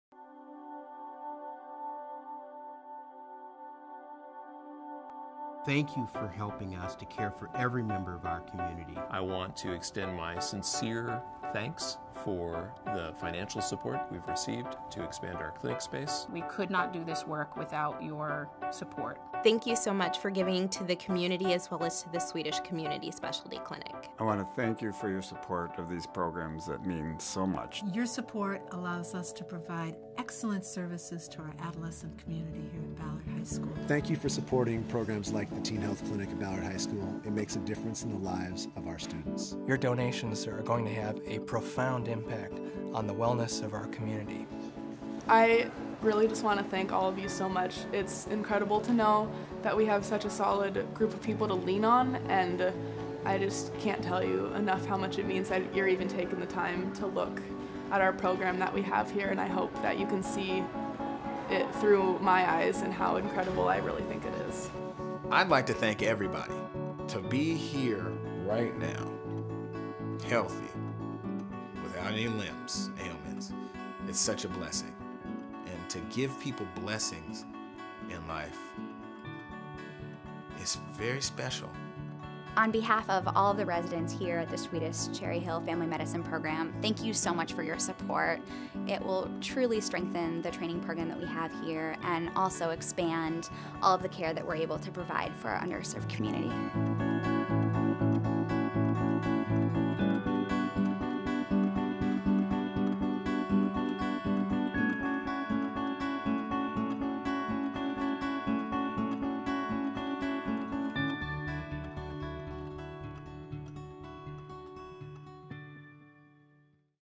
Thank you for supporting Destination Swedish! (HD)
Thanks to everyone who supported the Destination Swedish luncheon on Feb. 11, 2014 at the Seattle Sheraton Hotel! The event raised more than $920,000 to support Innovative Solutions for Healthier Communities.